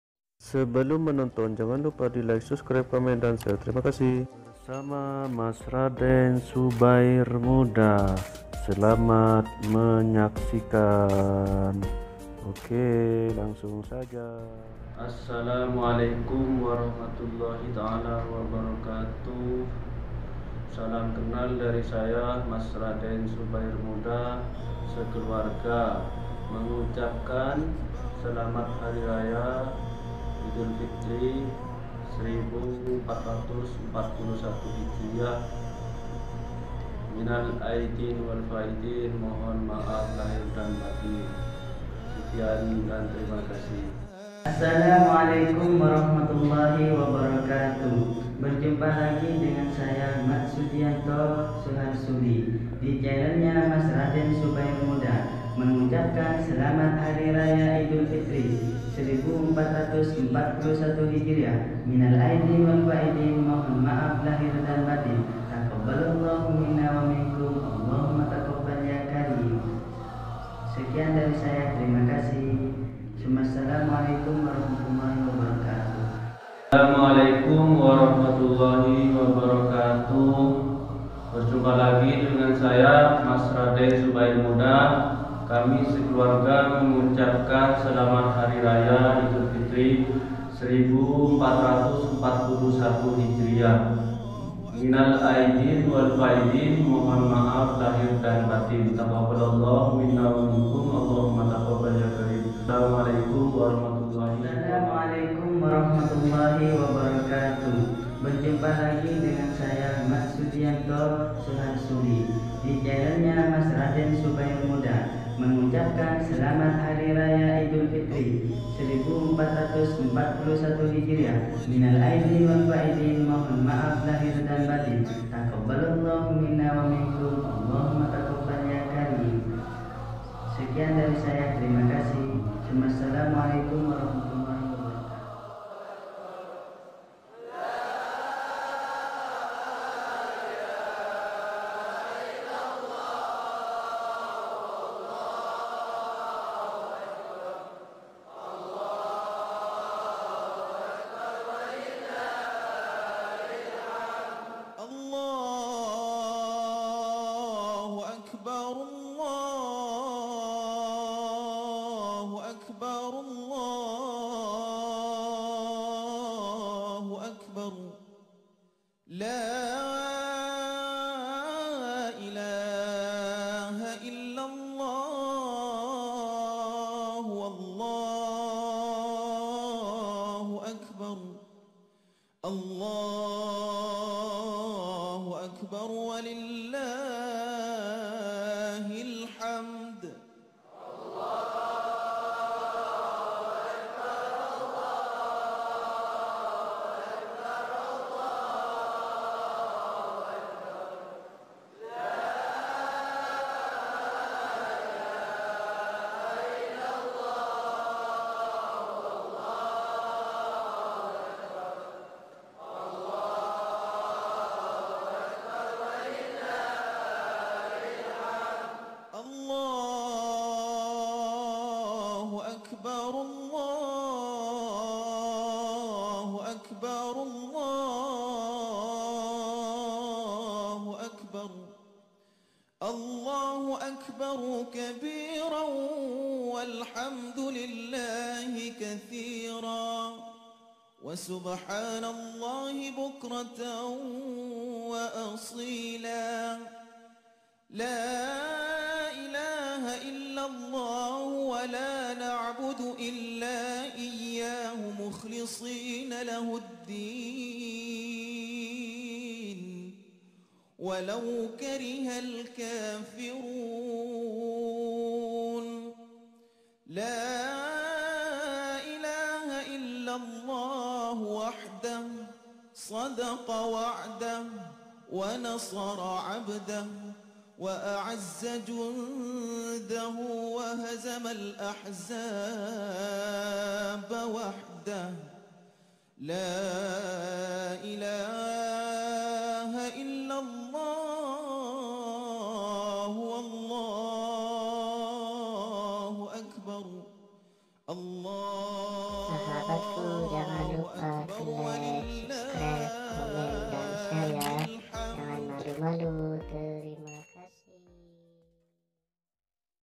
Selamat Hari Raya idul Fitri 1441 Hijriyah 2020
di kesempatan yg mulia ini saya buat video Selamat Hari Raya idul Fitri 1441 Hijriyah 2020 dari Mas Rhaden SubairMuda sekeluarga.
Mengucapkan
Minal Aidzin walfaidzin
Mohon ma'af lahir dan batin